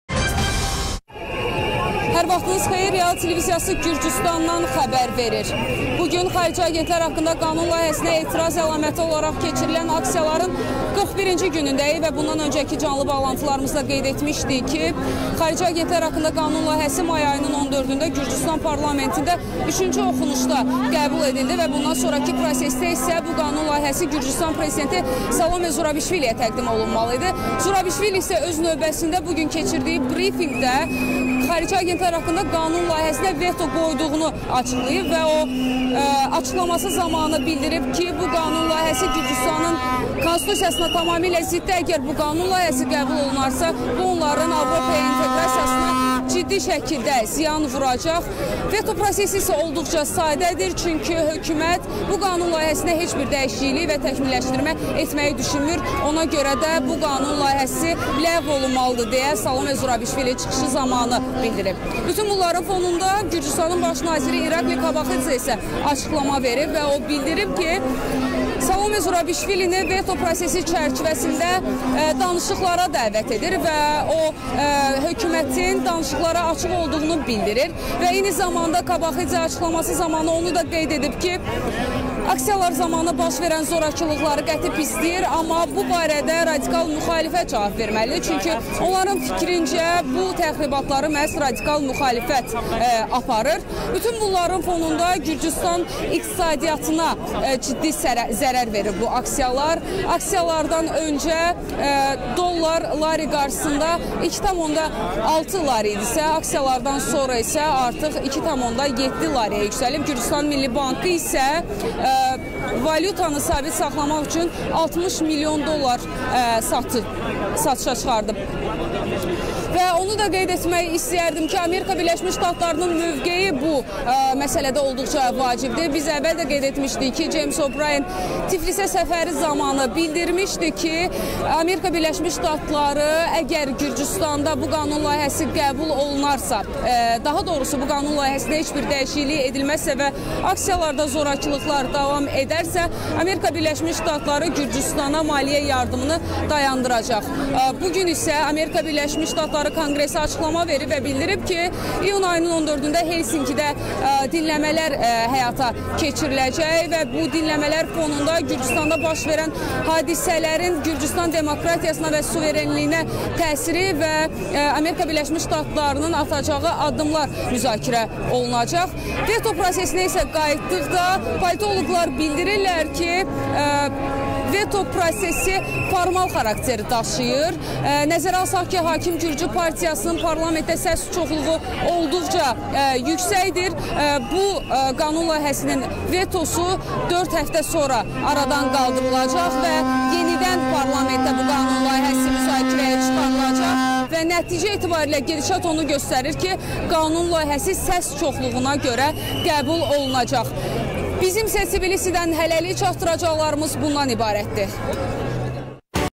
Gürcüstanda aksiyaların 41-ci günü
Prezident S.Zurabişvili “xarici agentlər”haqqında qanuna veto qoydu